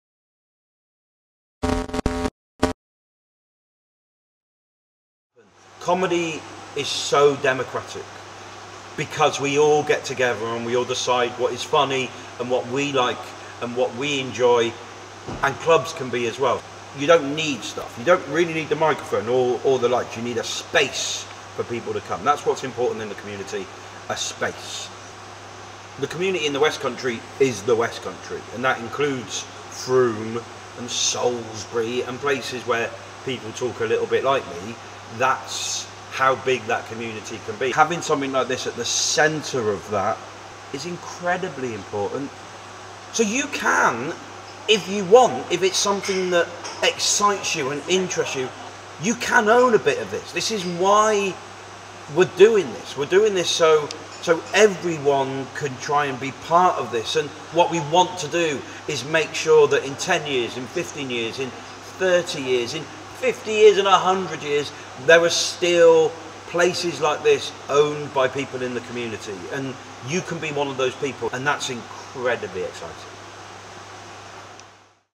Comedian Mark Olver on how YOU can own a piece of Komedia Bath!
Fancy a piece of this?! Comedian, compere and all round lovely man Mark Olver chats about how YOU can become a co-owner of Komedia Bath and why it is SO very important that we all make this happen together!

Head over to our crowdfunder page to invest, donate & find out more!